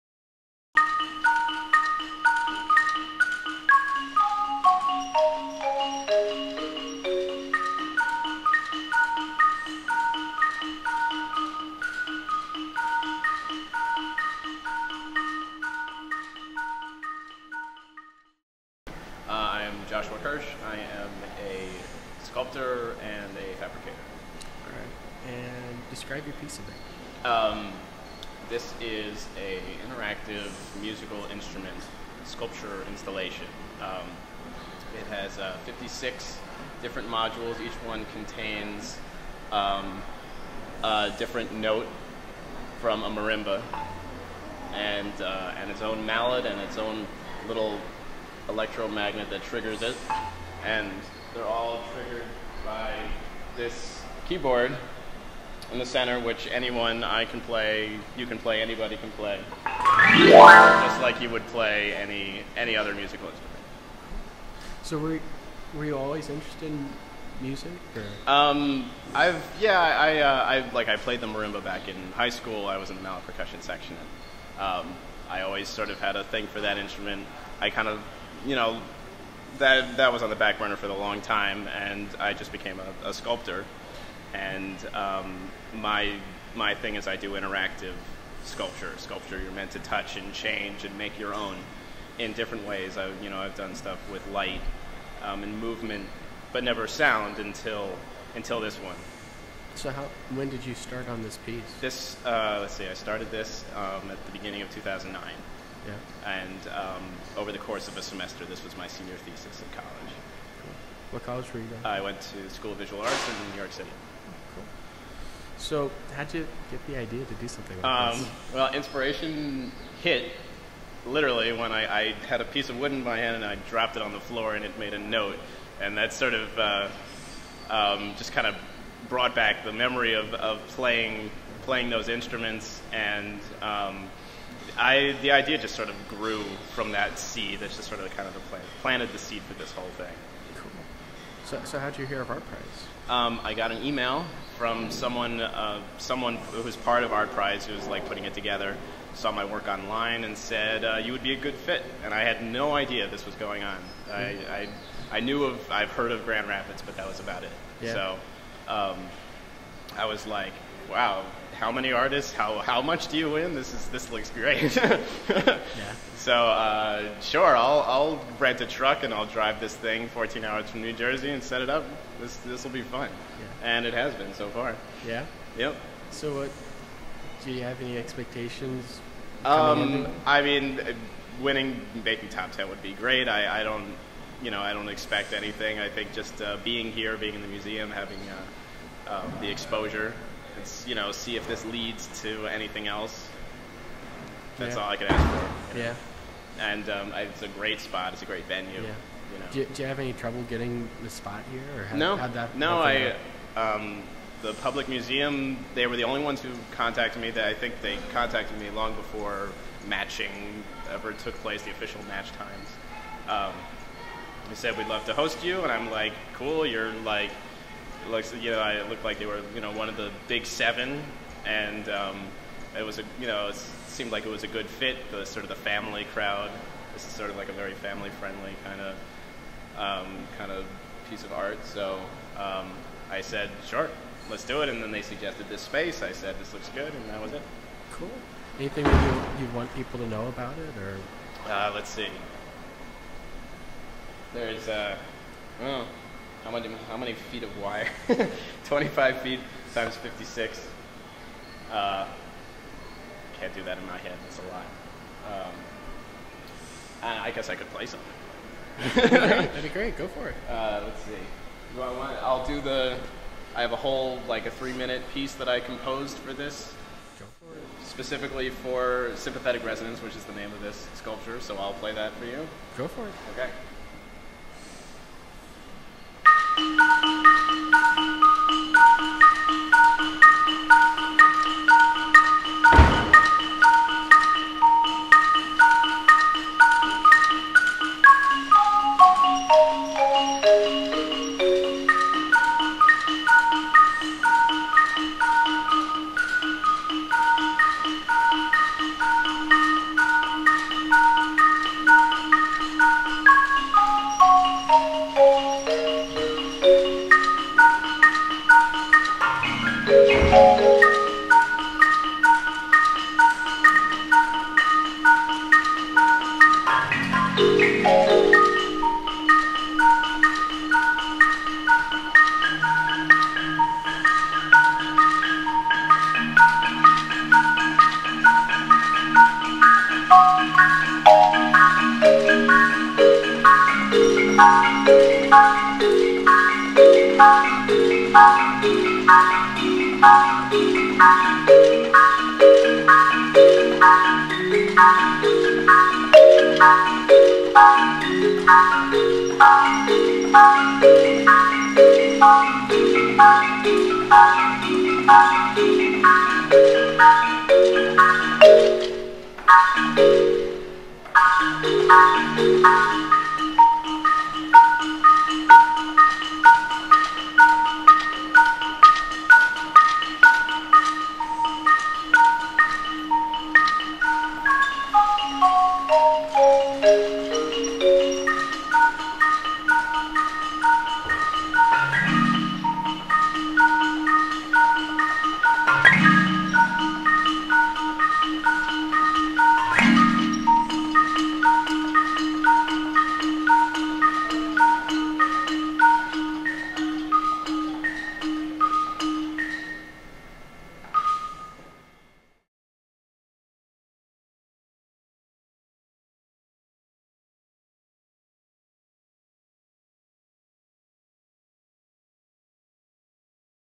Joshua Kirsch ArtPrize Interview
This is an interview that I did with ArtPrize artist Joshua Kirsch, at the Grand Rapids Public Museum on the eve of ArtPrize.  His entry is an interactive musical piece.  Be sure to give him a vote up!  His number is 47541.

for all things ArtPrize.